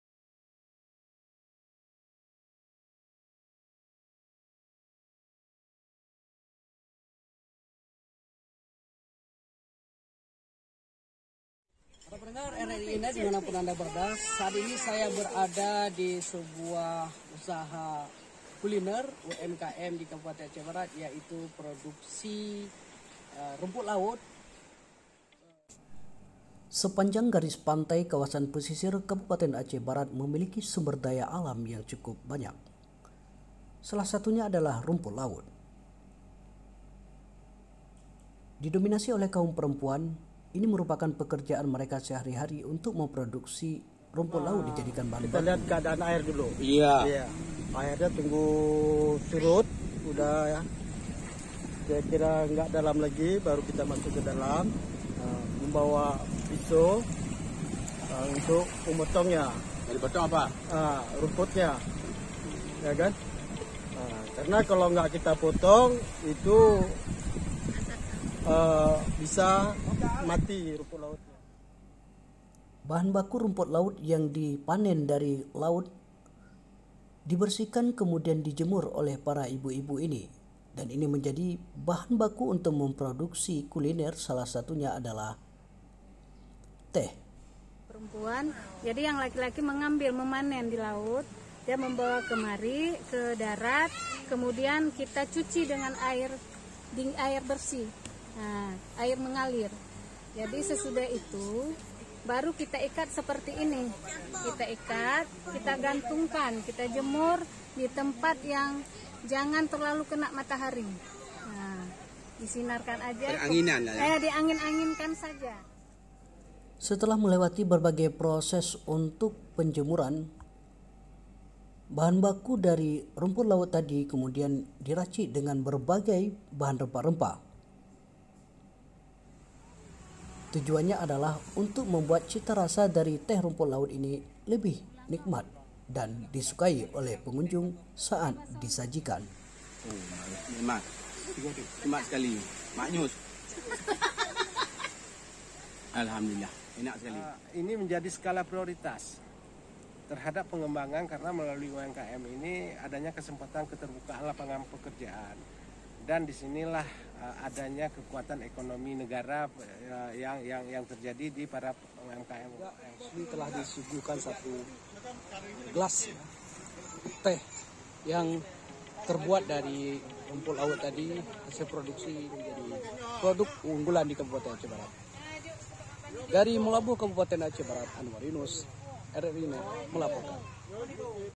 Potensi Daerah : Rumput Laut Untuk olahan makanan di Lhok Bubon Aceh Barat
Rumput laut dikawasan ini diolah menjadi berbagai jenis makanan, salah satunya adalah teh rumput laut.